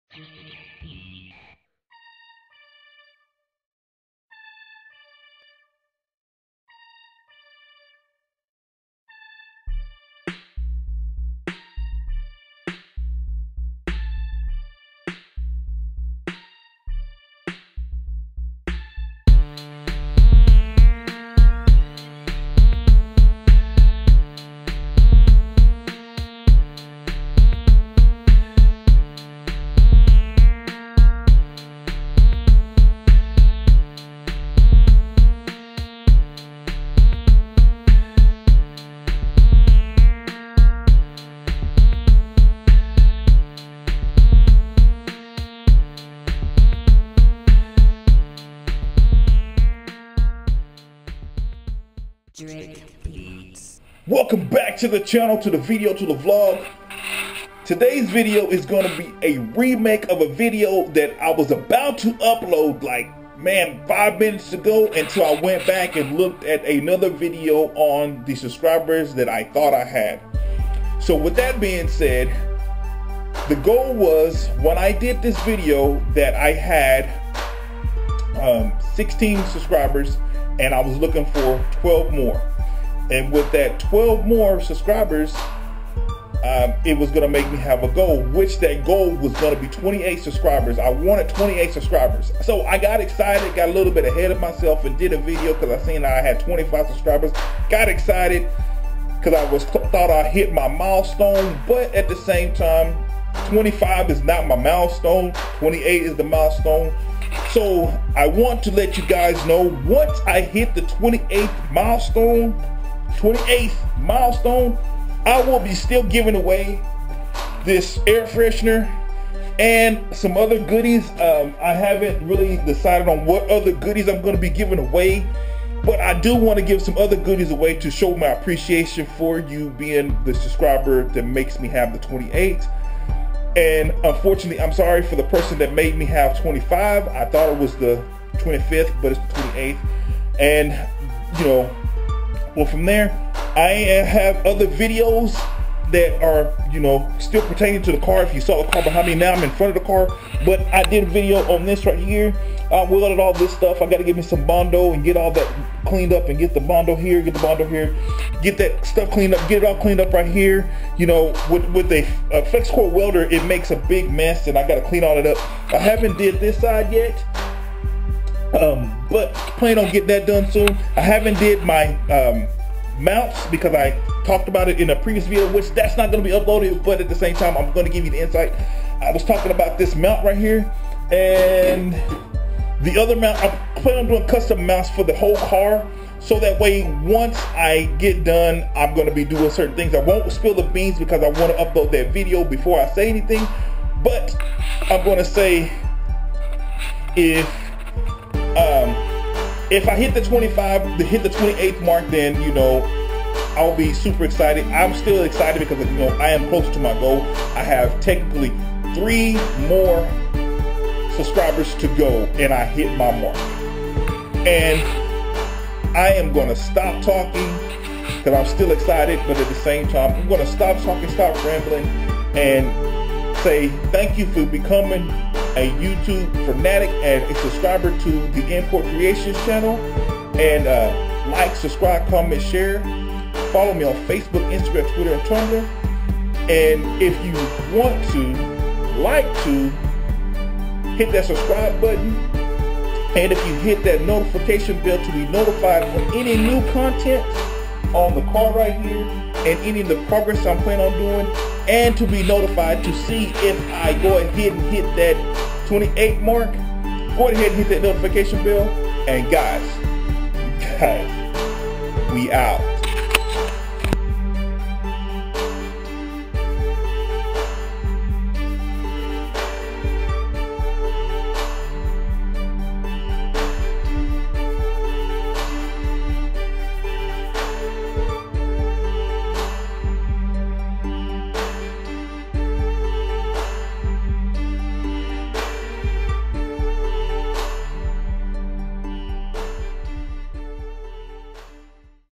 SO CLOSE TOO (IT)
So close to my goal that i got ahead of my self and made a video for the wrong thing

        But i still want to say thank you the sub i have now and to the
        one to come

Respect is as a follow !

                                                        HERE